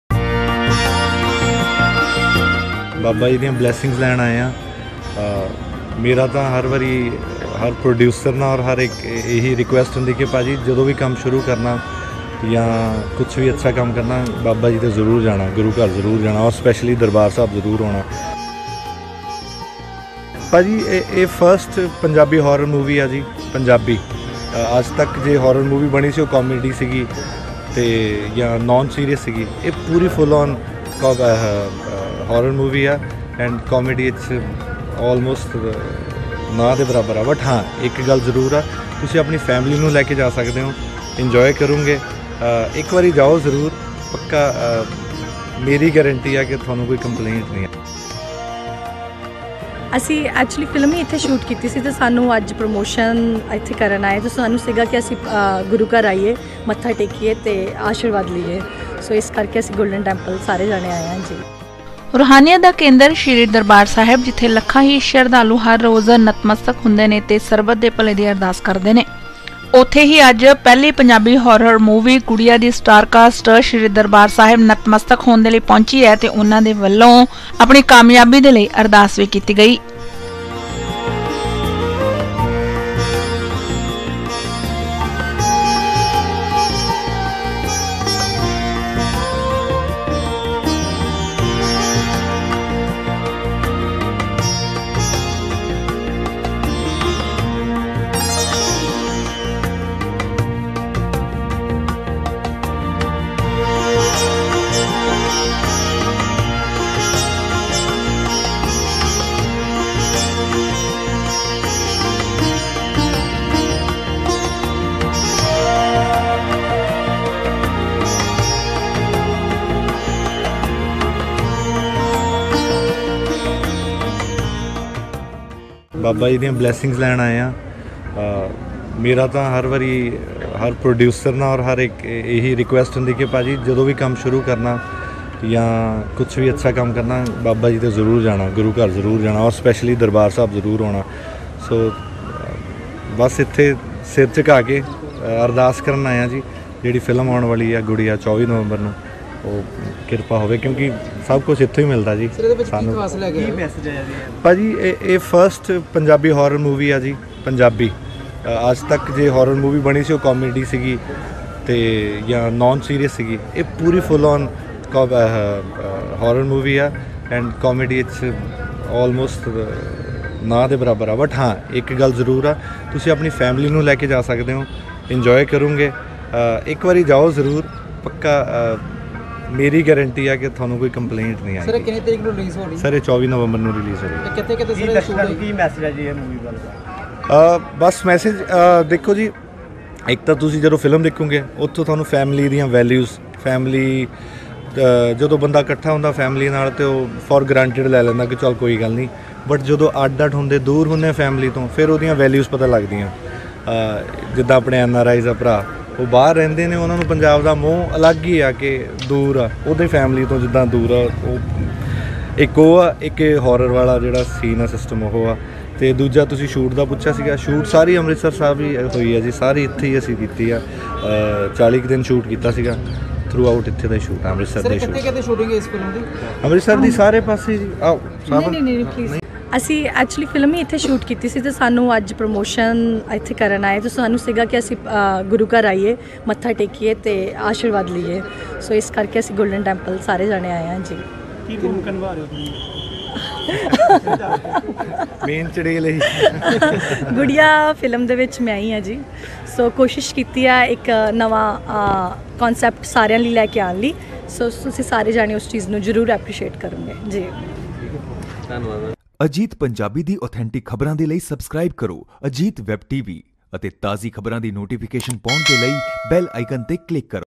Sri Harmandir Sahib ਨਤਮਸਤਕ ਹੋਣ ਪਹੁੰਚੀ ਪਹਿਲੀ ਪੰਜਾਬੀ Horror ਫ਼ਿਲਮ Gudiya ਦੀ Starcast
ਫਿਲਮ ਦੀ ਕਾਮਯਾਬੀ ਦੇ ਲਈ ਸੱਚਖੰਡ ਸ੍ਰੀ ਦਰਬਾਰ ਸਾਹਿਬ ਪਹੁੰਚ ਕੇ ਕੀਤੀ  ਅਰਦਾਸ